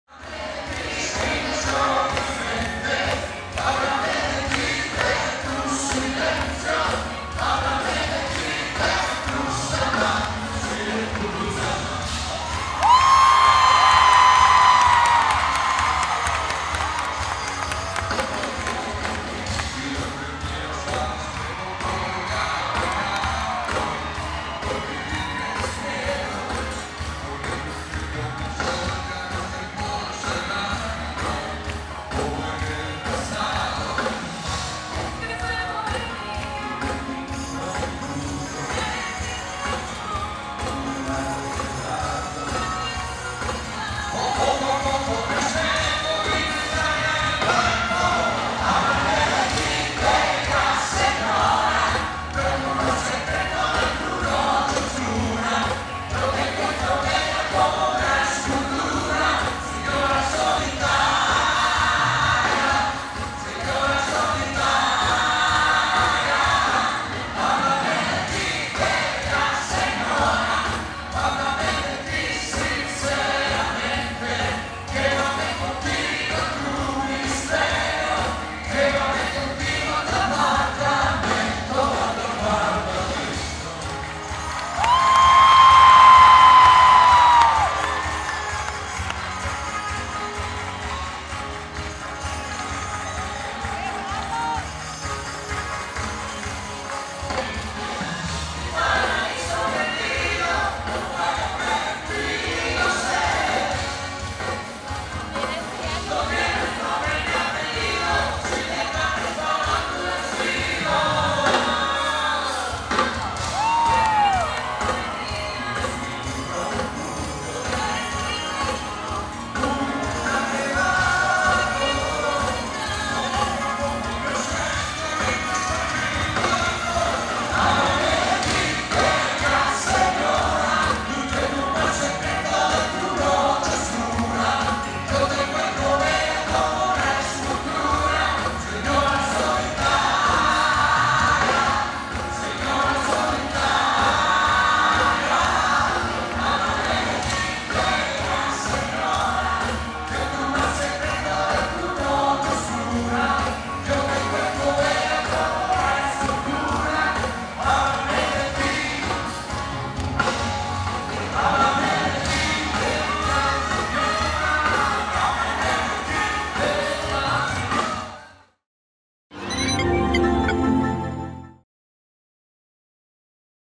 Emmanuel y Mijares conquistan el Palenque del FEX
Emmanuel y Mijares conquistan a su público con sus canciones y su entrega en el escenario del Palenque de FiestasdelSol